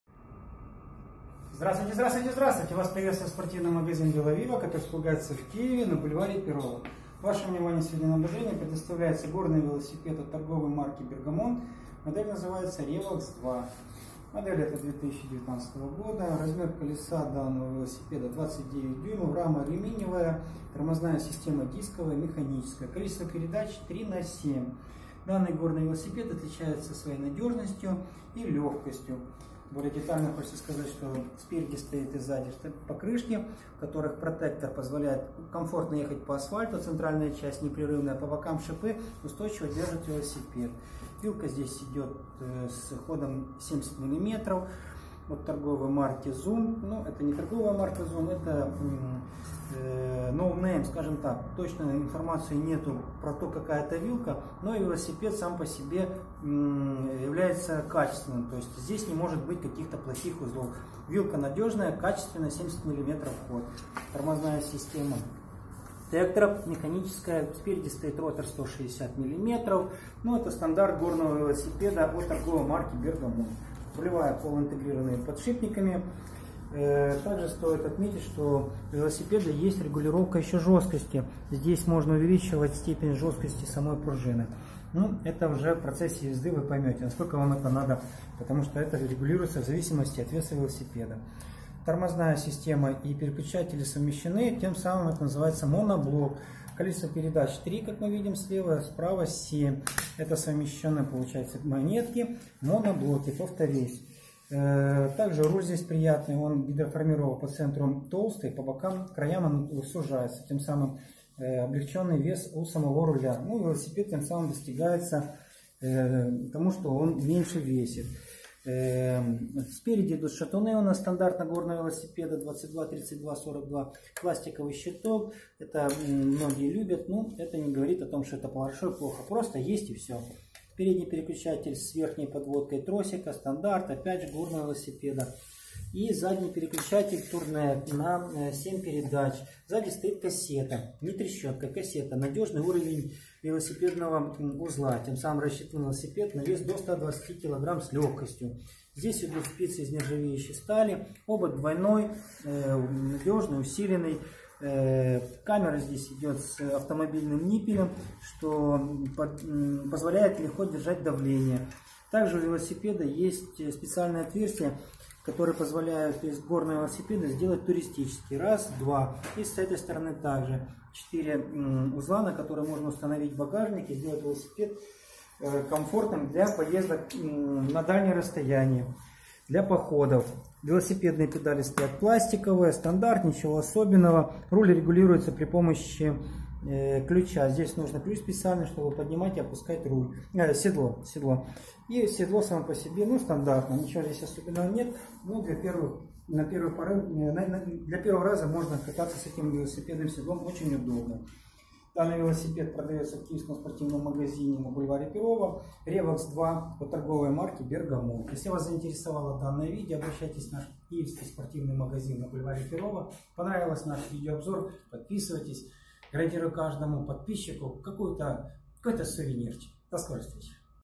Горный велосипед Bergamont Revox 2 (2019). Веломагазин VeloViva
Размер колеса: 29 | 27/5 дюймов;
Количество передач: 3Х7 speed;
Задний переключатель: Shimano RD-TY300;
Тормоза: дисковые механические;
Вилка: 100 мл. ход